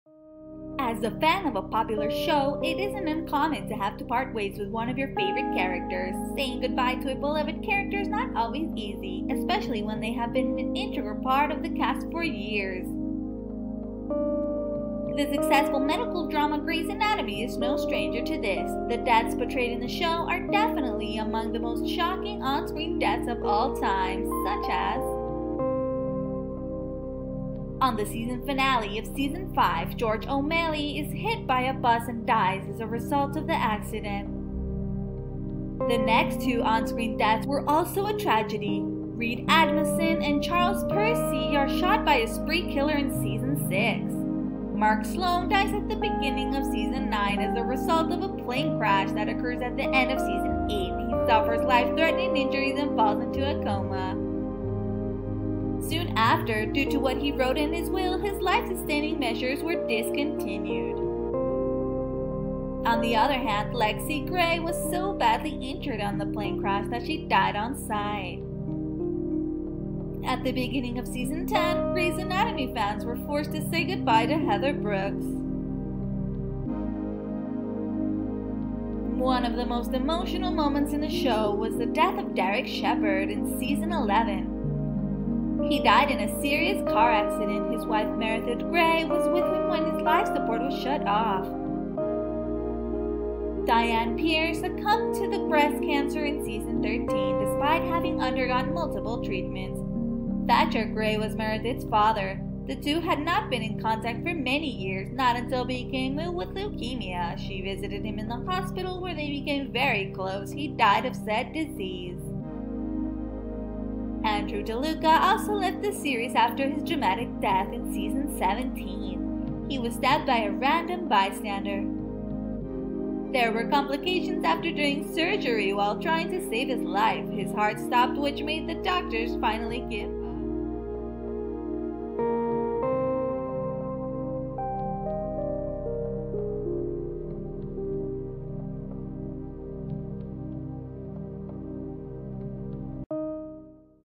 'Grey's Anatomy': The Beloved Characters That Died In The Show | Celebrity Hot Goss
The death of a character can hardly be avoided by screenwriters: sooner or later a popular character has to get killed off. Even in the successful medical drama Grey's Anatomy fans have had to say goodbye to some of their favorite characters.